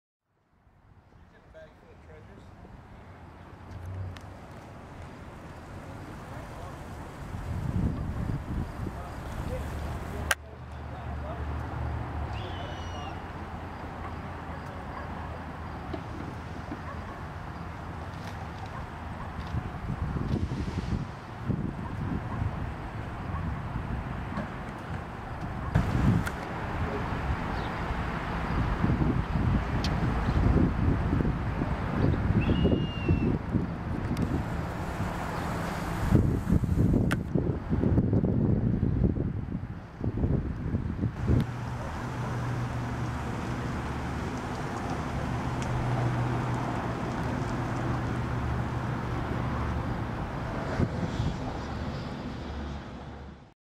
Hawks in the City
I photographed these juvenile red-tailed hawks on 26 August 2016 in Rapid City, South Dakota. The parent birds had been flying around above them but by the time I got my camera from my car the adult birds were elsewhere.

A man who worked at the Thrift Store seemed very proud of the hawks and told me they'd been watching them grow up through the summer. He is the one who said they had nested in the trees behind the buildings.

Sorry for the shaky video-- I didn't have my tripod with me and the telephoto magnifies any movement.